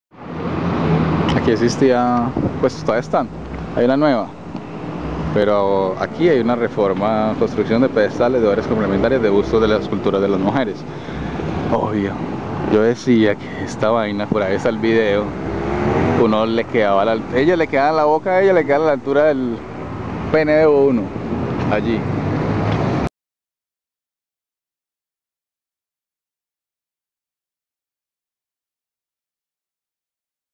Reforma de esculturas de mujeres en Medellín
Si has seguido este canal, por ahí (no sé dónde) hay una critica a unas esculturas de mujeres destacadas y poderosas de Antioquia, que fueron mal instaladas, pues era bustos en una placa de baja altura, y todo se sentía menos respetables y poderosas.

Menos mal repensaron la plaza, y han subido de altura las pobres esculturas.

Era el año 2011, no se vaya a ir a ver cómo va la obra.